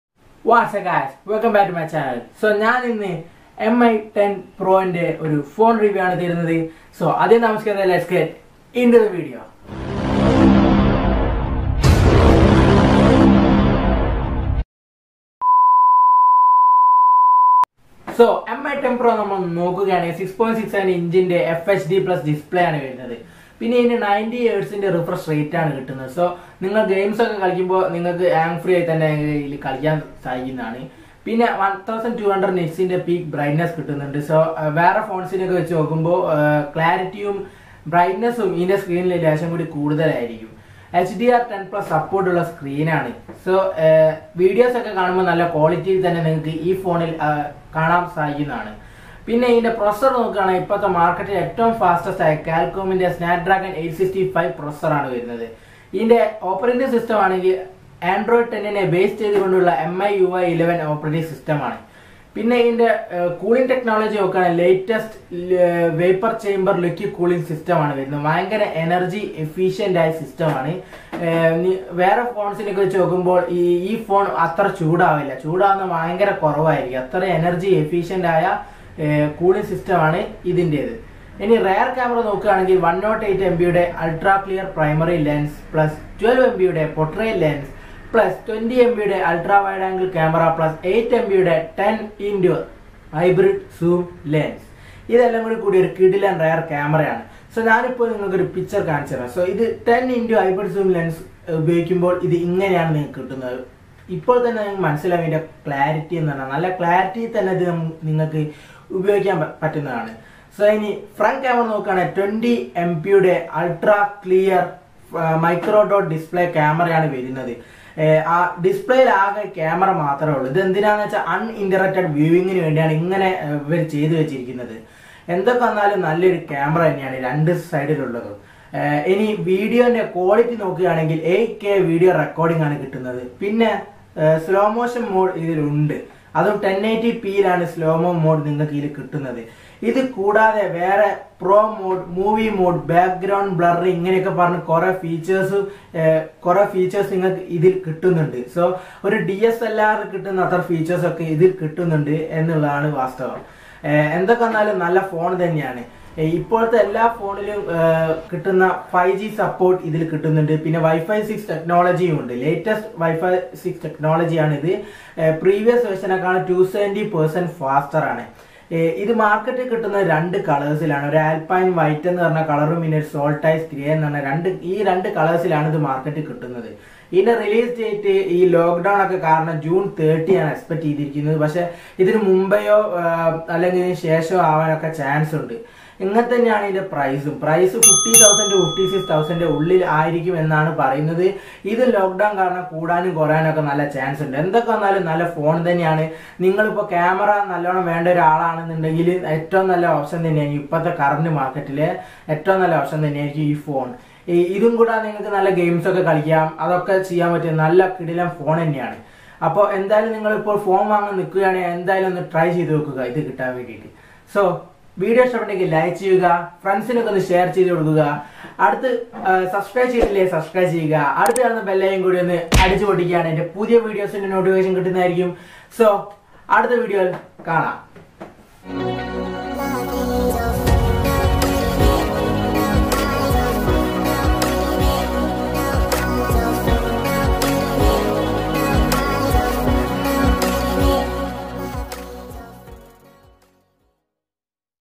Xiaomi Mi 10 Pro | specs and price in India (new 5G support, 8K Video recording)
The new Xiaomi Mi 10 Pro has various new features like 8k video recording , new 5G support,108 mp ultra clear primary lens , movie mode , vlog mode , pro mode , etc ..

It has a quad rear camera of 108 mp ultra clear lens + 20 mp ultra wide angle camera + 12 mp protrait  lens + 8mp 10x Hybrid zoom lens . IT has a 20 mp micro dot display camera ,6.67 inch fhd+ display with 90Hz refresh rate , 1200 nits peak brightness and hdr 10 + support